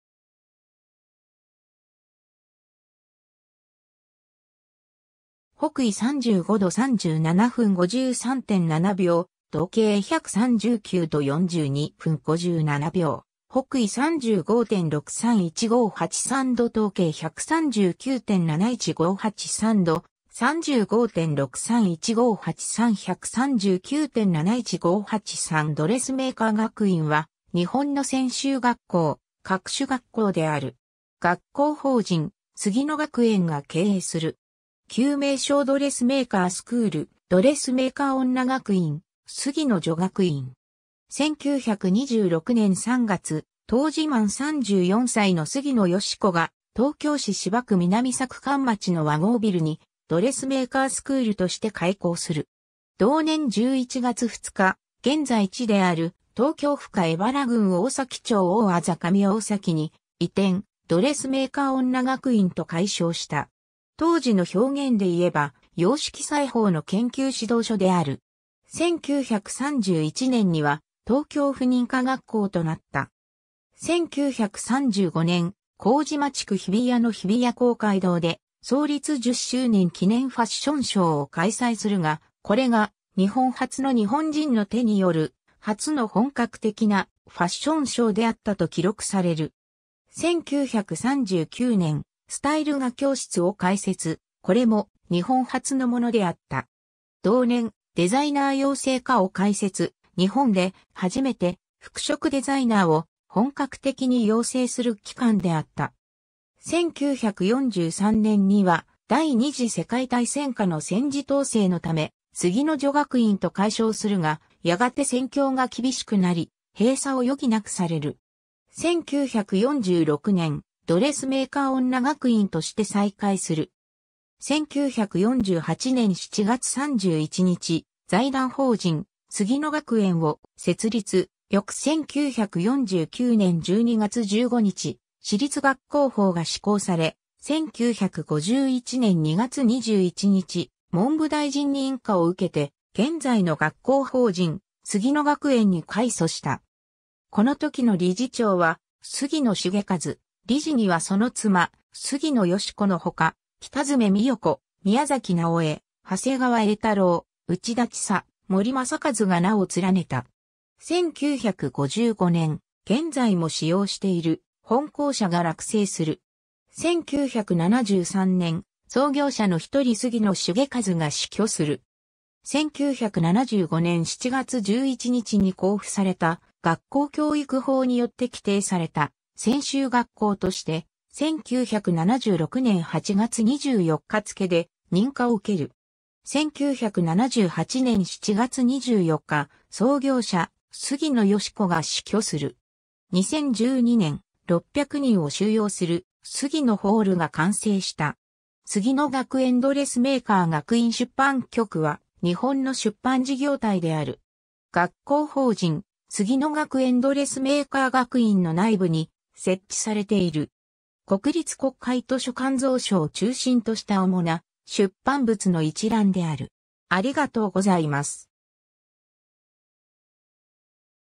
ドレスメーカー学院
北緯35度37分53.7秒 東経139度42分57秒 / 北緯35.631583度 東経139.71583度 / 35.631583; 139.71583 ドレスメーカー学院（ドレスメーカーがくいん）は、日本の専修学校、各種学校である。
学校法人杉野学園が経営する。
旧名称ドレスメーカー・スクール（英語: Dressmaker School）、ドレスメーカー女学院（ドレスメーカーじょがくいん）、杉野女学院（すぎのじょがくいん、第二次世界大戦中）。
 1926年（大正15年）3月、当時満34歳の杉野芳子が、東京市芝区南佐久間町の和合ビル（現在の東京都港区西新橋、建物は現存せず）に「ドレスメーカー・スクール」として開校する。
同年11月2日、現在地である東京府下荏原郡大崎町大字上大崎（現在の東京都品川区上大崎4-6-19）に移転、「ドレスメーカー女学院」と改称した。
当時の表現で言えば「洋式裁縫の研究指導所」である。
1931年（昭和6年）には、東京府（現在の東京都）認可学校となった。
 1935年（昭和10年）、麹町区日比谷（現在の千代田区日比谷公園）の日比谷公会堂で、「創立十周年記念ファッションショー」を開催するが、これが日本初の日本人の手による初の本格的な「ファッションショー」であったと記録される。
1939年（昭和14年）、「スタイル画教室」を開設、これも日本初のものであった。
同年、「デザイナー養成科」を開設、日本で初めて「服飾デザイナー」を本格的に養成する機関であった。
1943年（昭和18年）には、第二次世界大戦下の戦時統制のため「杉野女学院」と改称するが、やがて戦況が厳しくなり、閉鎖を余儀なくされる。
 1946年（昭和21年）、「ドレスメーカー女学院」として再開する。
1948年（昭和23年）7月31日、「財団法人杉野学園」を設立、翌1949年（昭和24年）12月15日、私立学校法が施行され、1951年（昭和26年）2月21日、文部大臣（現在の文部科学大臣）認可を受けて、現在の学校法人杉野学園に改組した。
このときの理事長は杉野繁一、理事にはその妻杉野芳子のほか、北爪巳代子、宮崎ナヲエ（宮﨑直江）、長谷川映太郎（鎌倉書房社長）、内田チサ（双葉洋裁女塾、現在の福岡デザイン専門学校初代理事長）、森政一（湖山社、現在の松柏社創業社長）が名を連ねた。
 1955年（昭和30年）、現在も使用している「本校舎」が落成する。
 1973年（昭和48年）、創業者のひとり杉野繁一が死去する。
1975年（昭和50年）7月11日に交付された学校教育法によって規定された専修学校として、1976年（昭和51年）8月24日付で認可を受ける（設置年月日）。
1978年（昭和53年）7月24日、創業者・杉野芳子が死去する。
 2012年（平成24年）、600人を収容する「SUGINO HALL」が完成した。
 杉野学園ドレスメーカー学院出版局（すぎのがくえんドレスメーカーがくいんしゅっぱんきょく）は、日本の出版事業体である。
学校法人杉野学園ドレスメーカー学院の内部に設置されている。
 国立国会図書館蔵書を中心としたおもな出版物の一覧である。